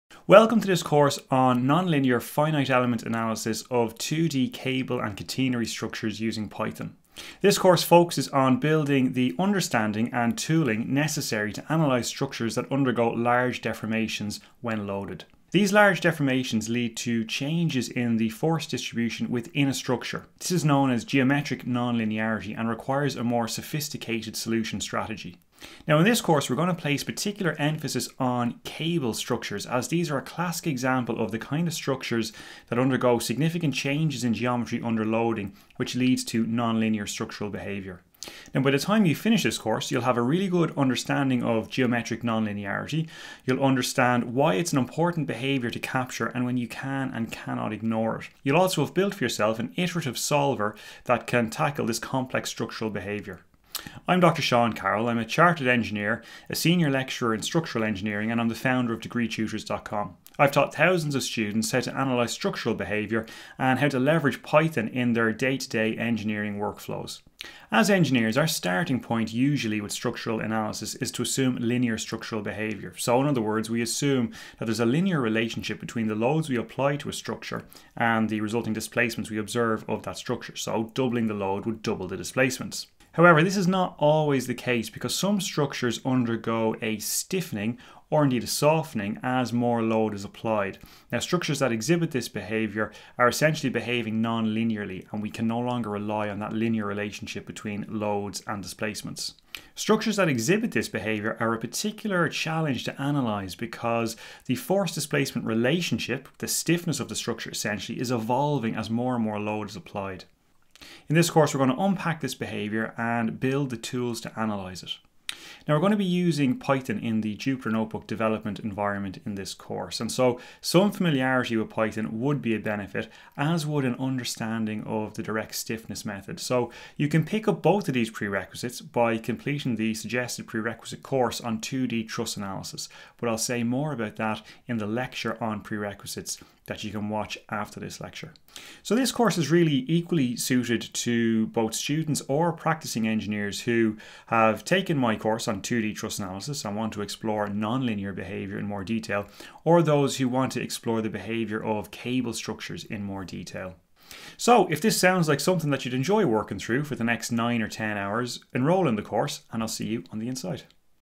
New Course | Non-linear finite element analysis of catenary & cable structures in Python
This is a course preview - check out the full course over on…DegreeTutors.com…

FYI - this is a members-only course. If that’s not something you’re interested in, no problem, stick around here on Youtube - I’ll have more free tutorials and projects ready soon.

In the course…
✅ You’ll learn about geometric non-linearity and when and why it should be considered.
✅ You’ll build a toolbox in Python for analysing non-linear cable structures that undergo large deflections
✅ You’ll use Blender, an open-source modelling tool, to quickly generate the initial structural geometry.